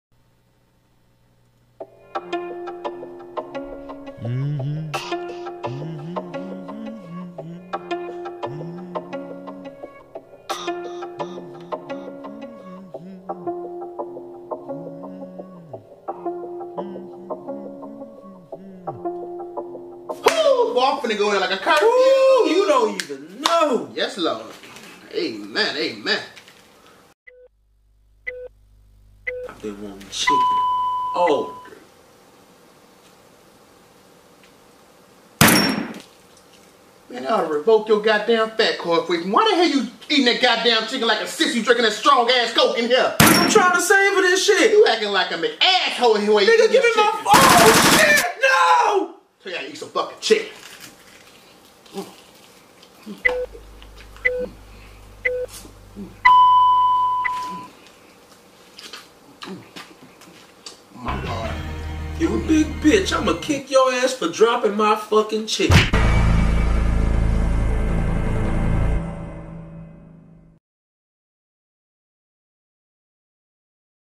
10 Mins or Less Presents: "Chicken Wings and Fat Boys"
Is there a proper way to eat a fried chicken wing? Well according to @yung_c and @chef247comedy there maybe. what happens when you get two fat boys in a chicken wing situation? itsa247hustle 10minorless comedy sketch chickenwings fatboy @mofo.shots